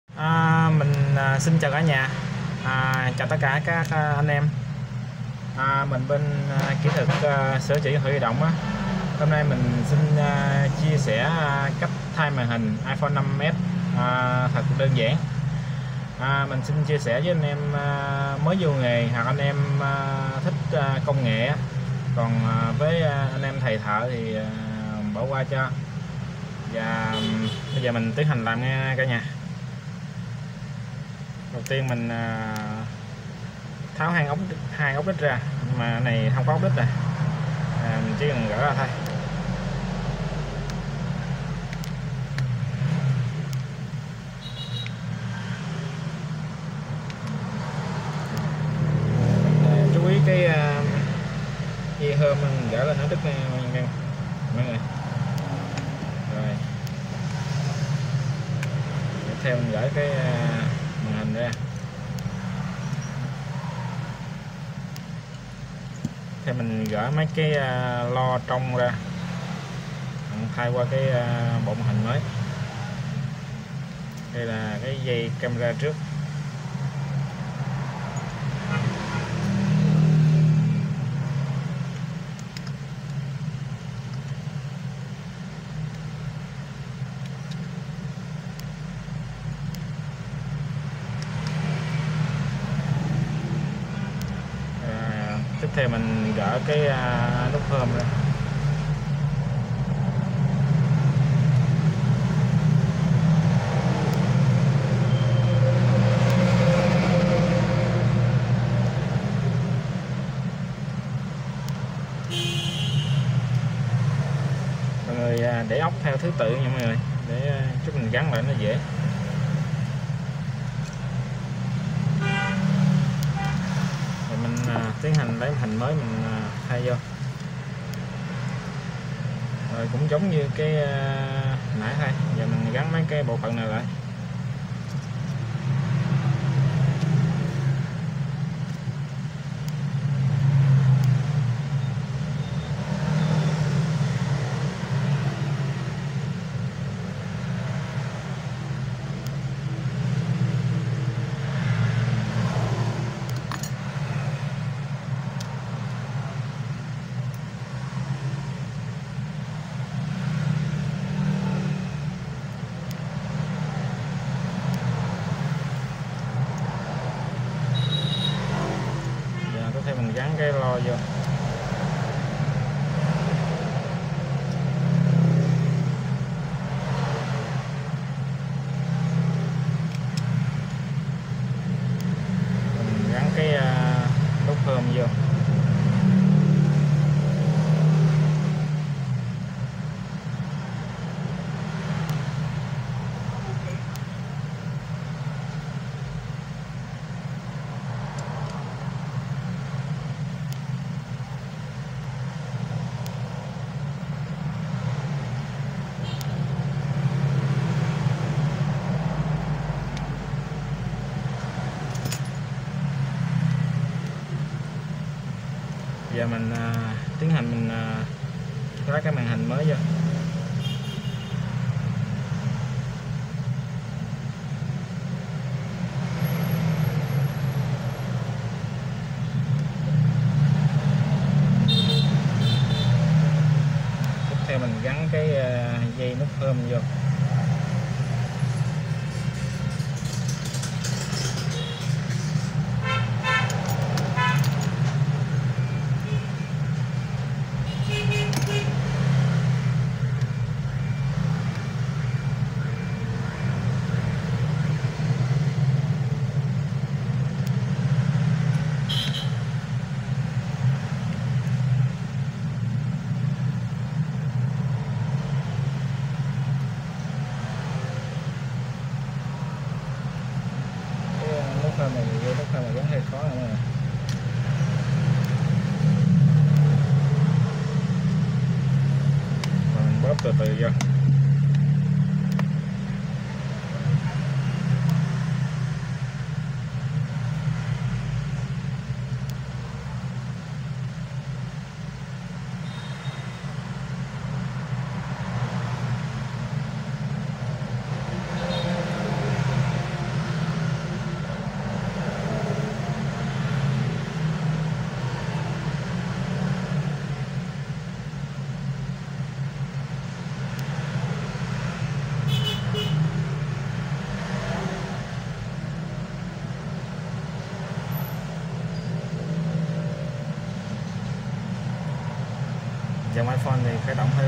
chia sẻ thay màn hình iphone siêu nhanh
kinh doanh mobi tv-hôm nay xin chia sẻ cách thay màn hình iphone với tất cả  anh mới vào nghề,xin chúc cả nhà vui vẻ bình an hạnh phúc..